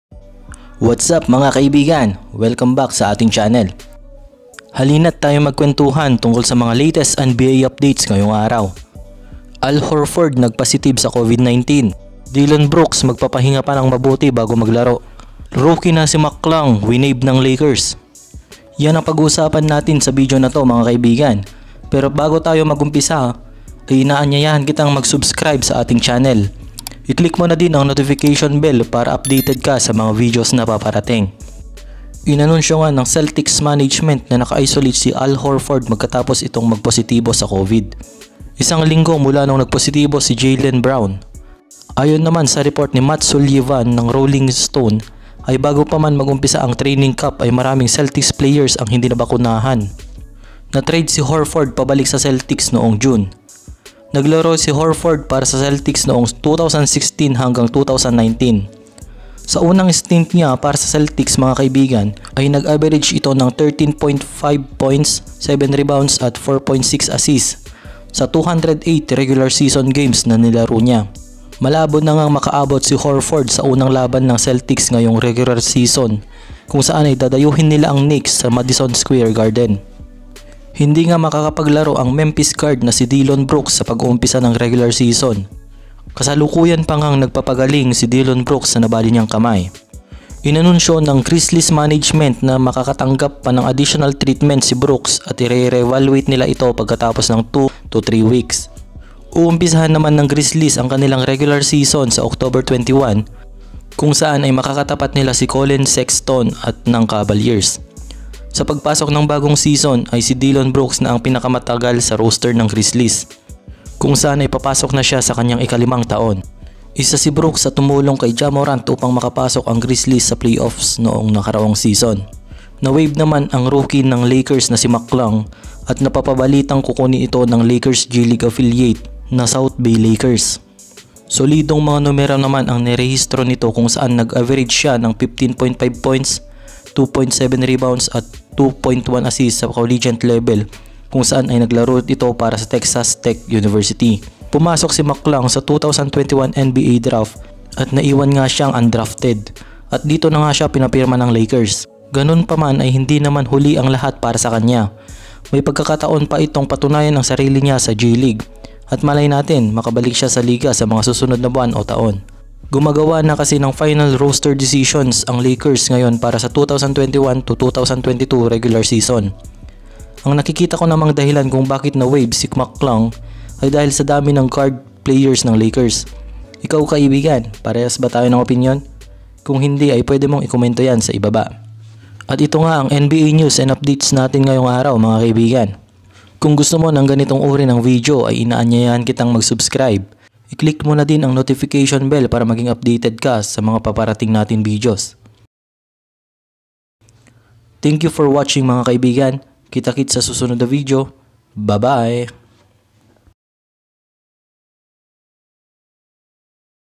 COVID POSITIVE SI AL HORFORD? | DILLON BROOKS MAGPAPAHINGA MUNA | LAKERS ROOKIE NAWAIVED?
Halinat ating pag usapan ang mga maiinit na balita ngayon araw.